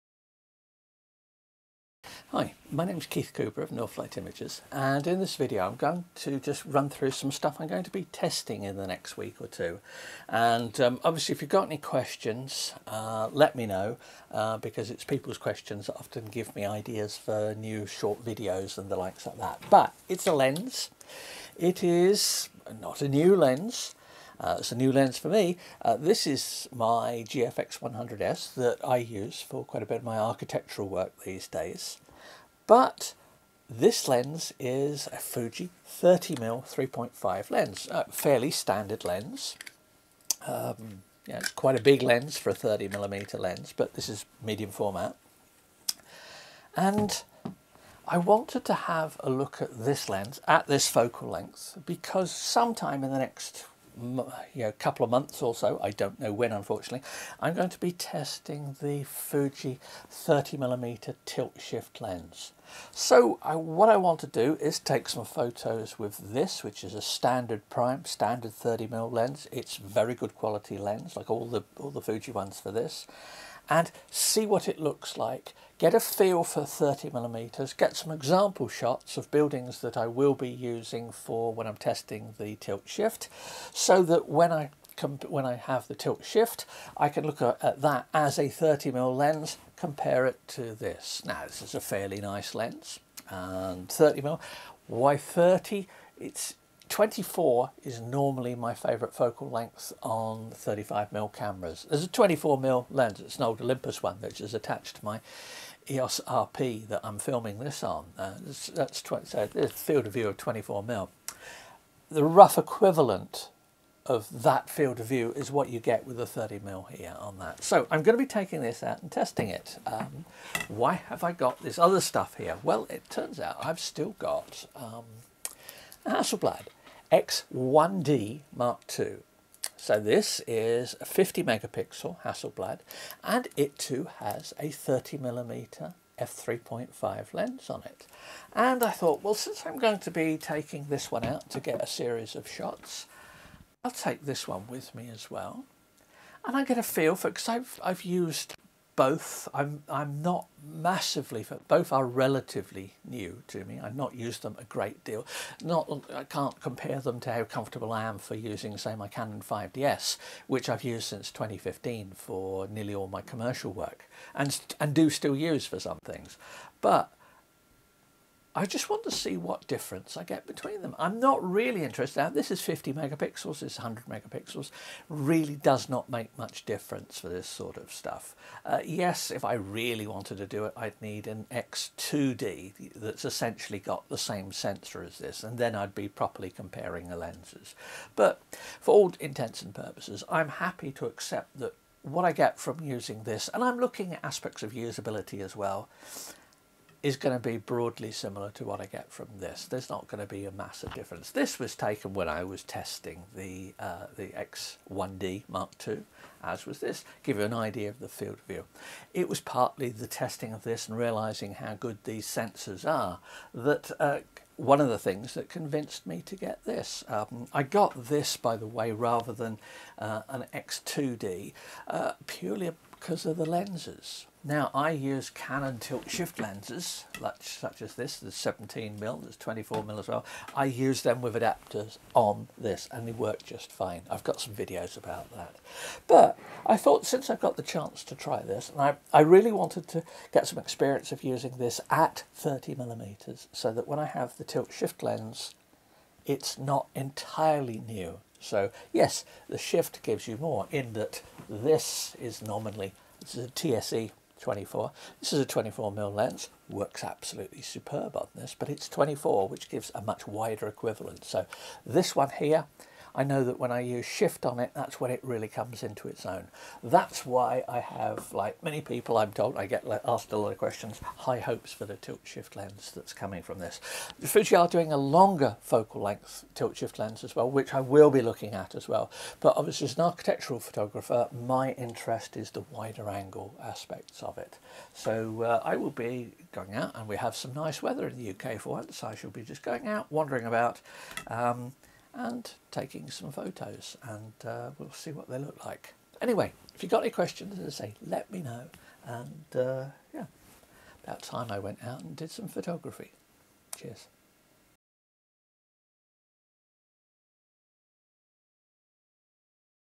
30mm primes for medium format - Fujifilm and Hasselblad 30/3.5. testing & camera comparison preview
[Preview] Testing  the Fujifilm 30mm f/3.5 prime lens on the GFX100 - getting a feel for 30mm before testing the Fujifilm 30mm tilt/shift lens.  How does the 30mm on the GFX100S compare to the XCD30/3.5 on an X1Dii?  Questions - AMA request

Since I've got the X1Dii here with a 30mm lens it seemed reasonable to take both out with me - Do the cameras change my approach to using 30mm on medium format?

My articles and videos are always free to access.
Any help with running this channel is gratefully received.
US Amazon photo/print gear

B&H Photo